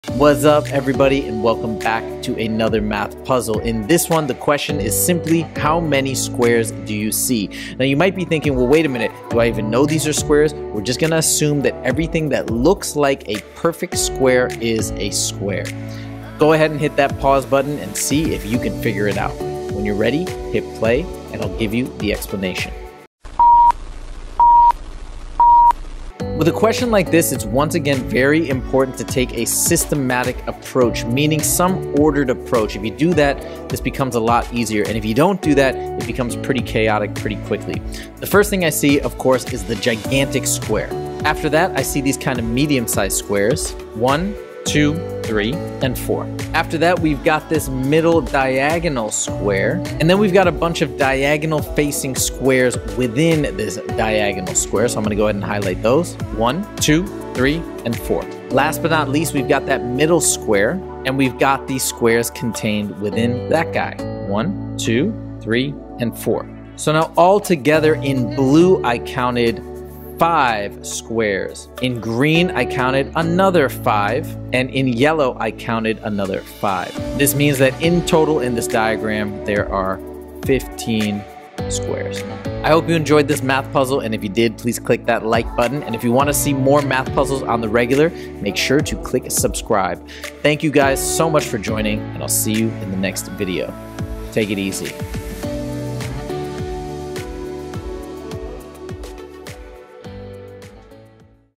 How many squares do you see?
This spatial reasoning problem is fun and visually stimulating! When you solve it, make sure to take a strategic approach. Check back every Friday for more awesome math puzzles!